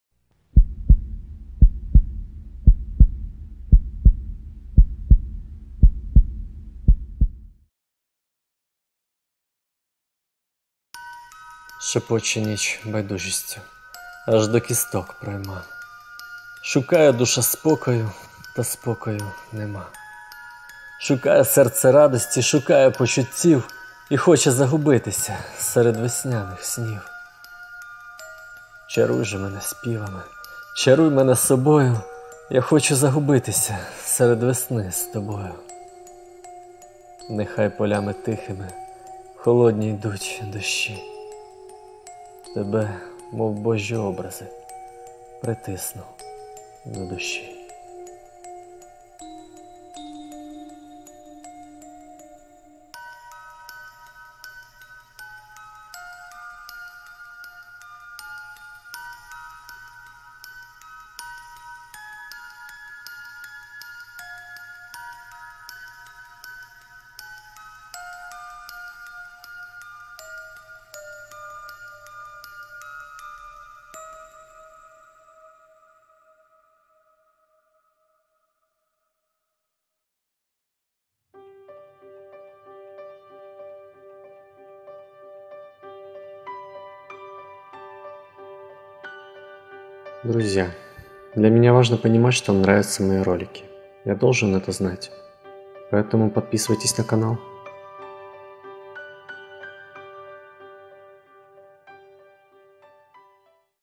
Автор Городинец Сергей "Шепоче ніч байдужістю" Стих
Каждую неделю на канале новое видео. Озвучиваю свои стихи и не только.
Оставьте свой комментарий под видео. Ваше мнение очень важно.

Шепоче ніч байдужістю, аж до кісток пройма
Шукає душа спокою та спокою нема

Шукає серце радості, шукає почуттів
І хоче загубитися серед весняних снів

Чаруй же мене співами, чаруй мене собою
Я хочу загубитися серед весни з тобою
Нехай полями тихими, холодні йдуть дощі
Тебе мов Божі образи, притисну до душі
Городинец Сергей ©